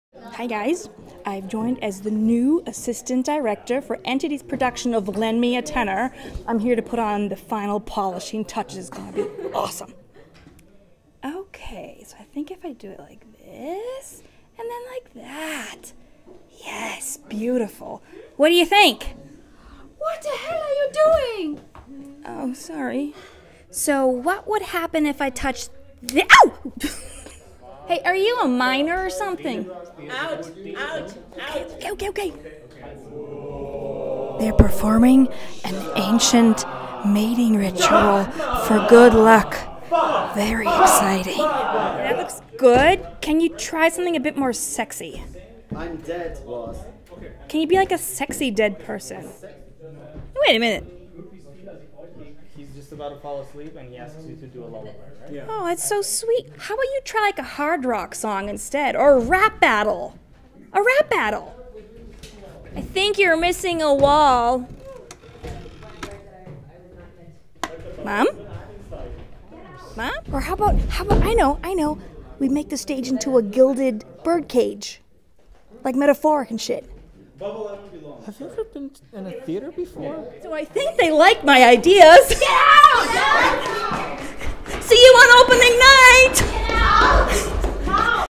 Behind the scenes of Ken Ludwig's Lend Me A Tenor
Guys, today is the big day! The opening night of Entity's Lend Me A Tenor! Elizabeth Goetze went undercover during dress rehearsal to see how the show is going. or maybe to stir up some trouble 😂 See for yourself.